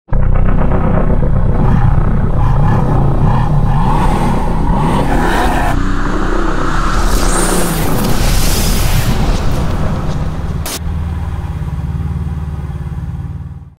Mercedes AMG GT Motion Poster
Full CG pitch film for the AMG GT in Motion Poster format. Work was done @ nhb  Studios Düsseldorf.
Christian Wallmeier
Senior 3D Artist - VFX Supervisor - Animation Director
cw3d.de